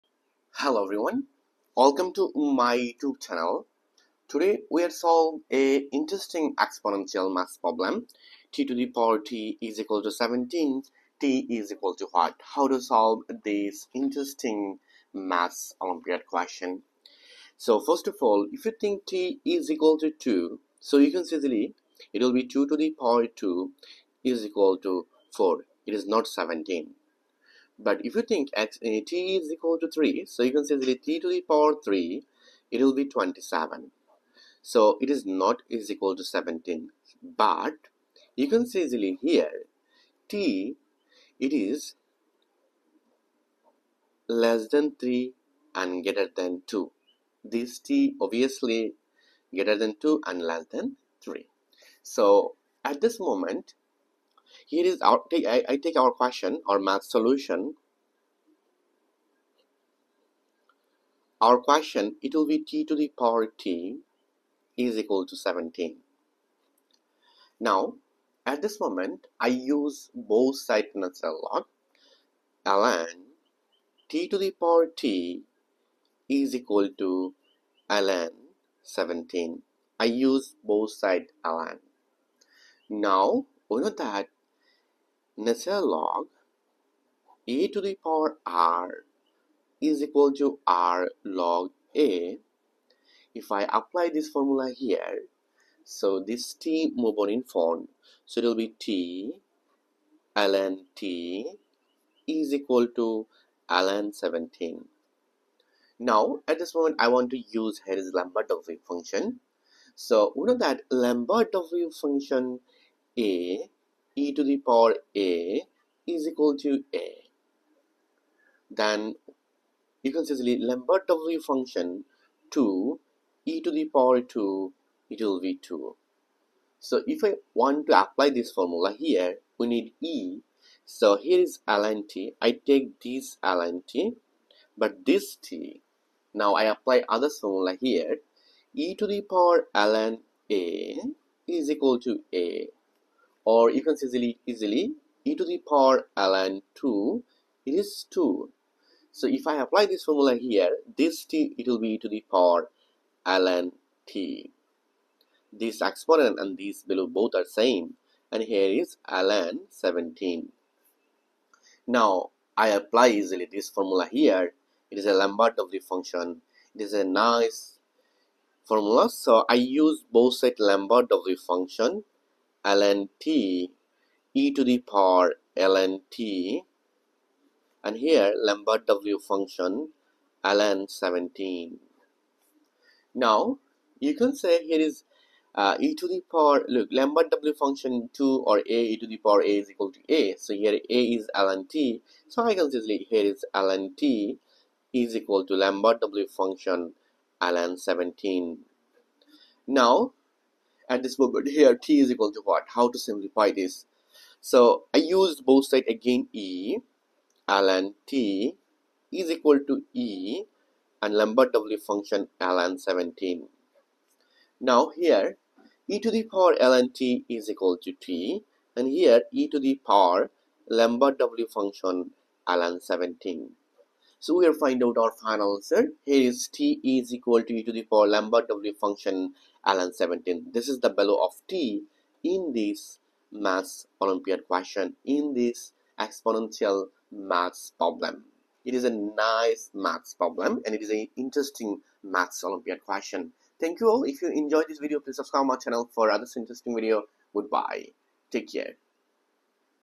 Harvard University entrance exam question | Olympiad algebra problem
Hello everyone ,Welcome to Rashel's classroom. In this video i solve a nice exponential expression. Find the value of t?